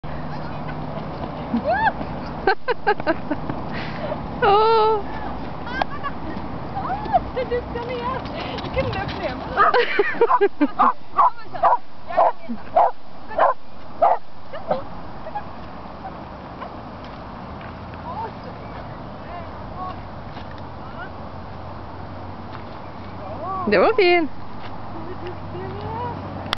Shanto and Tyra are spinning at the same time
A Ridgeback and a Sheltie are performing tricks together.